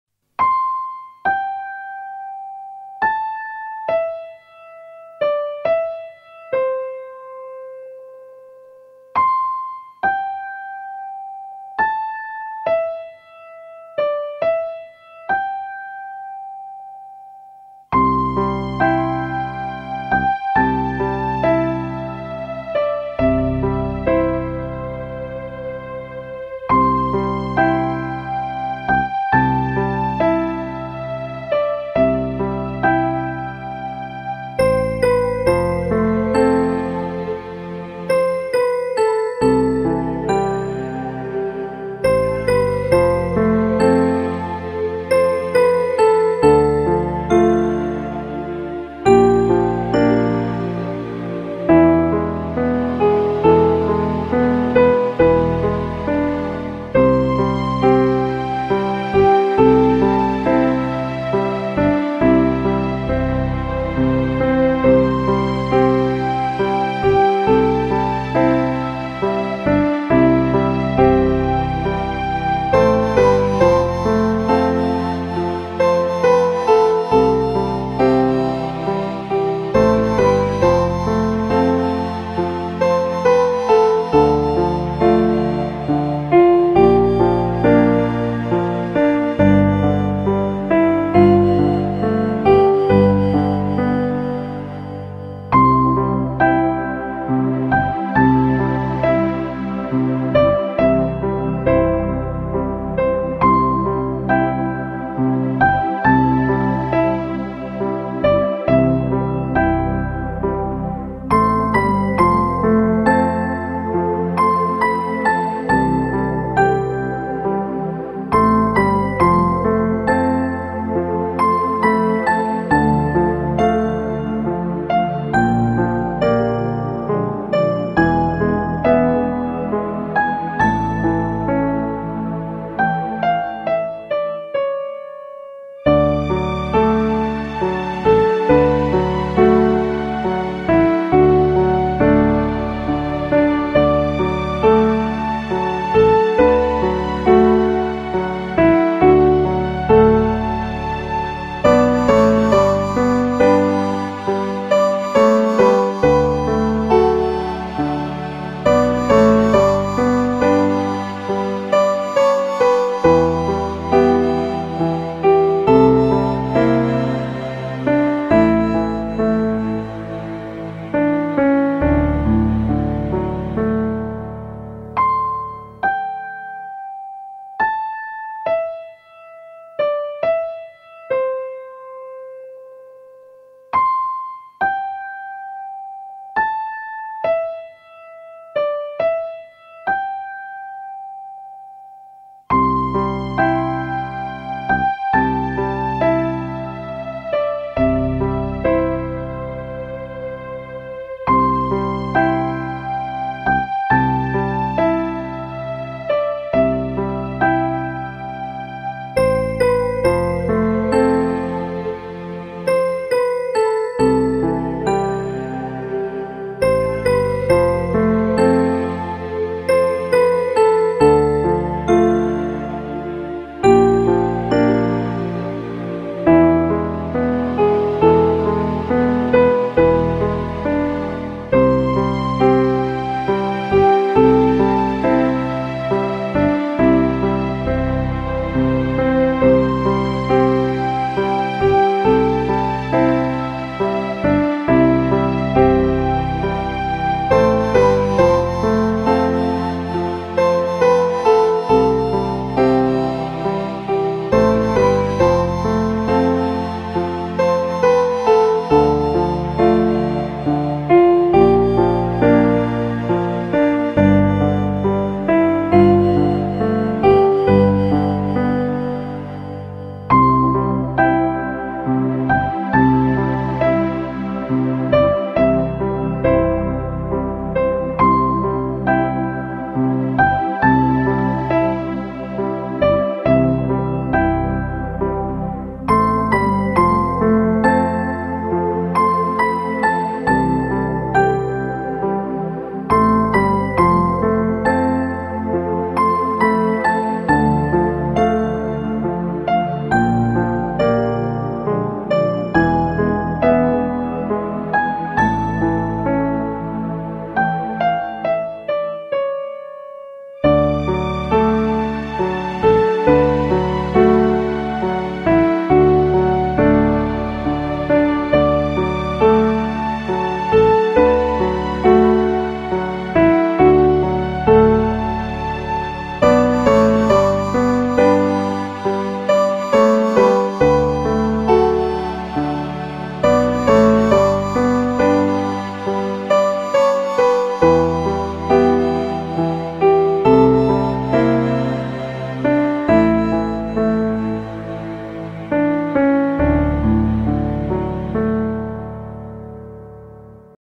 Lullaby for baby | Very beautiful music for babies | Will sleep fast!
Lullabies help babies to sleep good! They help regulate the emotions of the baby and also lullabies help establish a routine

It will help the further development of our channel🙏

Enjoy this beautiful lullaby 💤💖